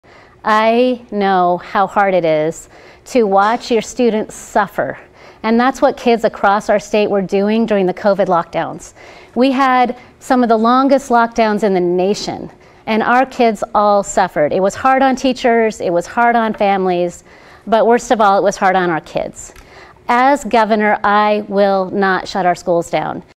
Christine Drazan: “As Governor, I Will Not Shut Our Schools Down”
The comments were made during the Oregon Gubernatorial Debate 10/19/22